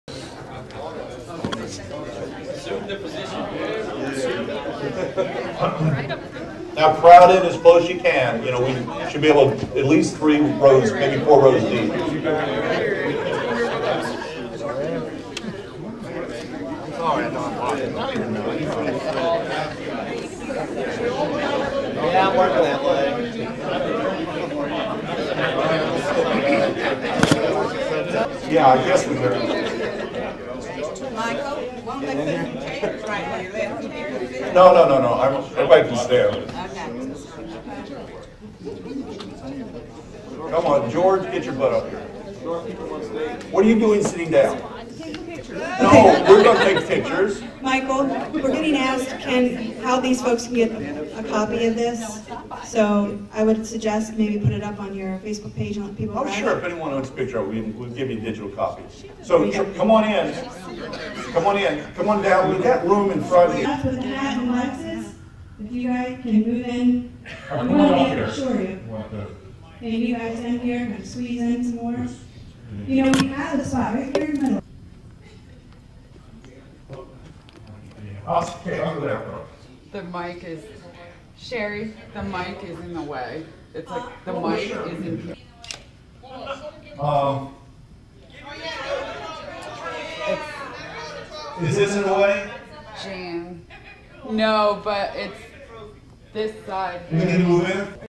Group Picture at Safety Technology 2015 Bootcamp
- Video of getting everyone together to take a group picture of the attendees at the Safety Technology 2015 Bootcamp